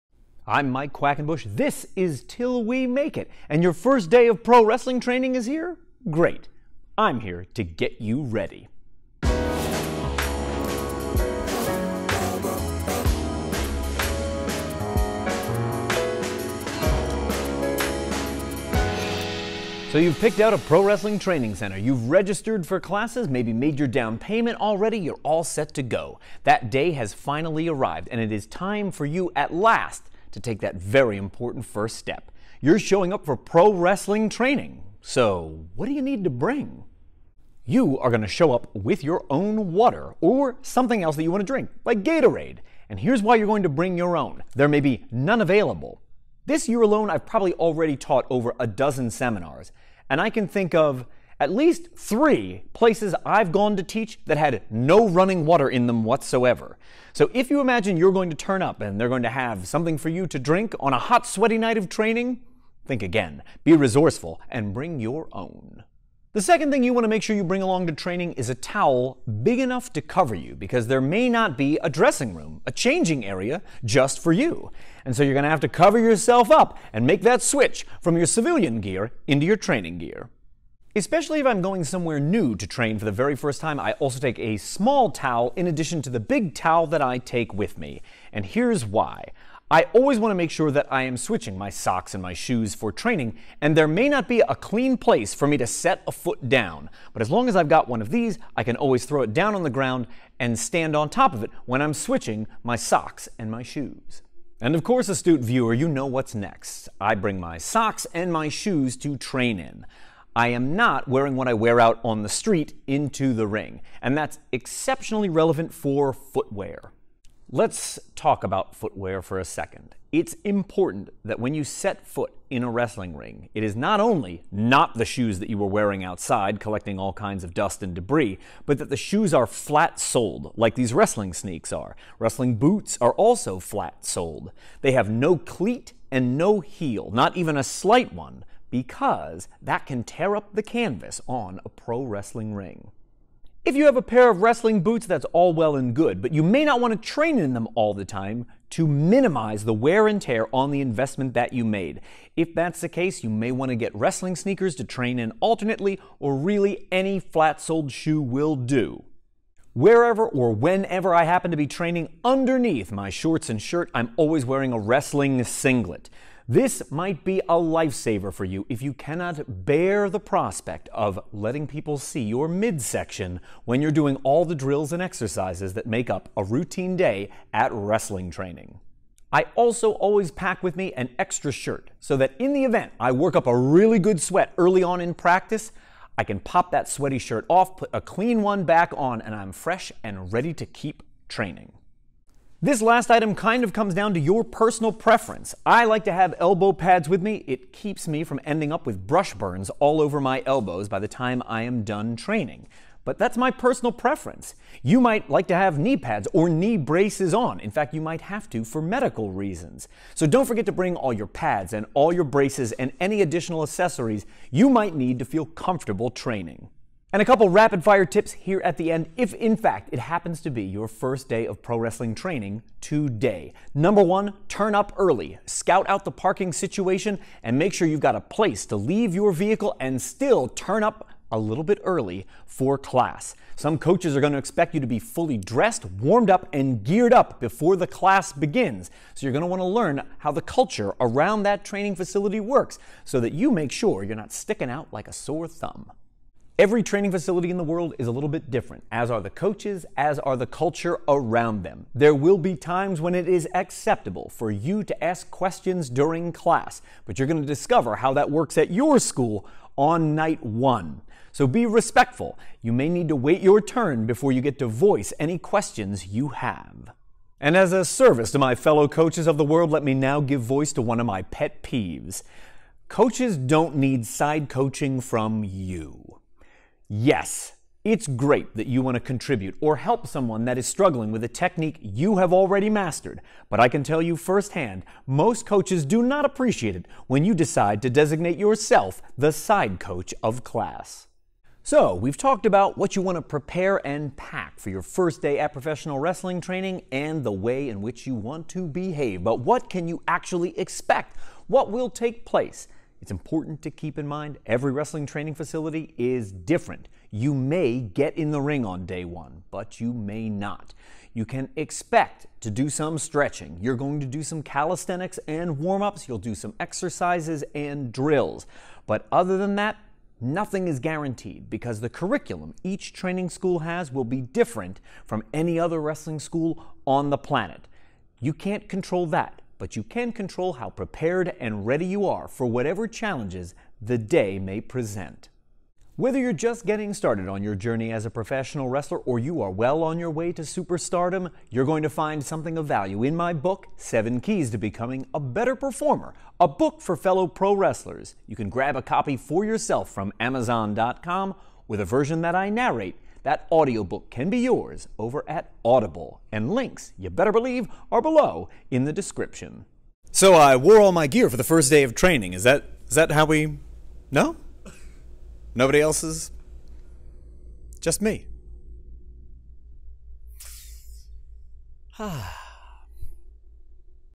Your FIRST DAY of Pro Wrestling Training!
I'm going to get you totally prepared for your first day of pro-wrestling training! Showing up prepared will help you overcome that natural degree of nervousness we all experience when starting a new venture, with people we've never met before.

The Wrestling Shoes I wear
I wear a Brute Wrestling Singlet
Great Elbow Pads
My favorite Knee Brace

My Lights
My Lavalier Mic